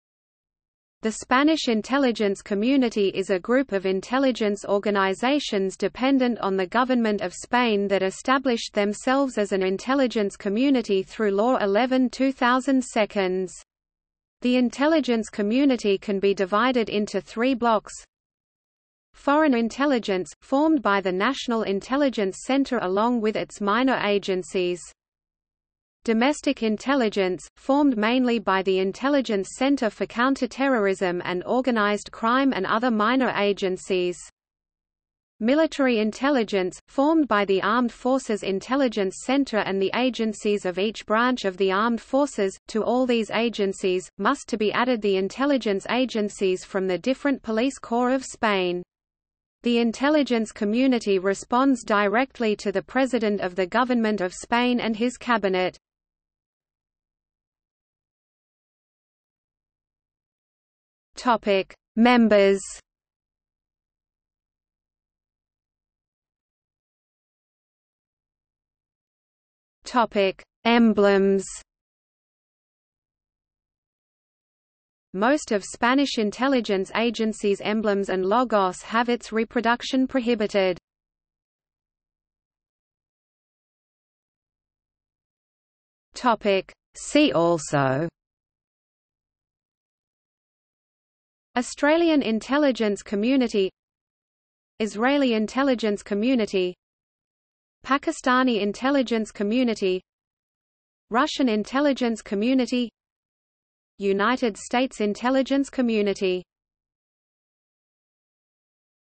Spanish Intelligence Community | Wikipedia audio article
00:00:57 1 Members
00:01:06 2 Emblems
00:01:22 3 See also

- Socrates

SUMMARY
The Spanish Intelligence Community is a group of intelligence organizations dependent on the Government of Spain that established themselves as an intelligence community through Law 11/2002. The intelligence community can be divided into three blocks:

Foreign intelligence: Formed by the National Intelligence Center along with its minor agencies.
Domestic intelligence: Formed mainly by the Intelligence Center for Counter-Terrorism and Organized Crime and other minor agencies.
Military intelligence: Formed by the Armed Forces Intelligence Center and the agencies of each branch of the Armed Forces.To all these agencies, must to be added the intelligence agencies from the different police corps of Spain. The Intelligence Community responds directly to the President of the Government of Spain and his Cabinet.